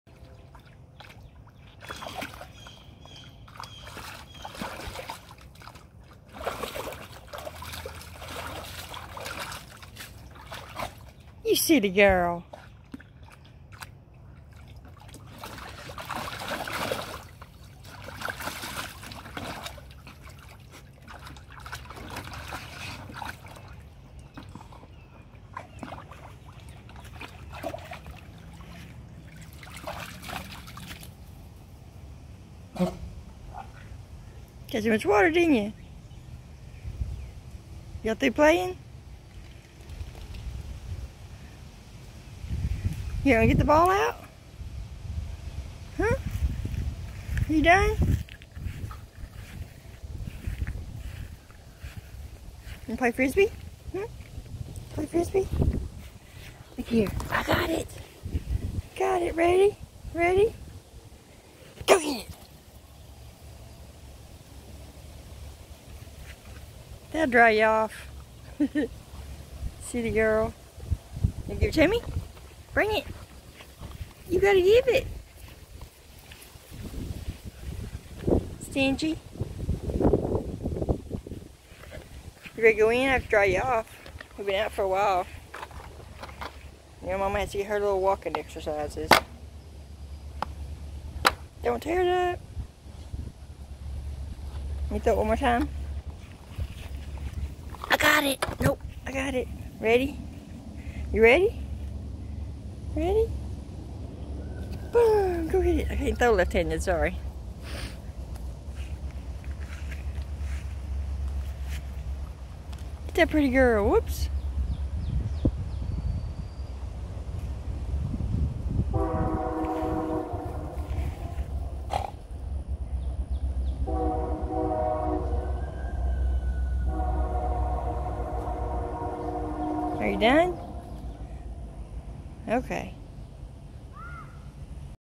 Boogie splashing & running
Boogie helping me get a little exercise. 😉😊
This baby girl loves to play!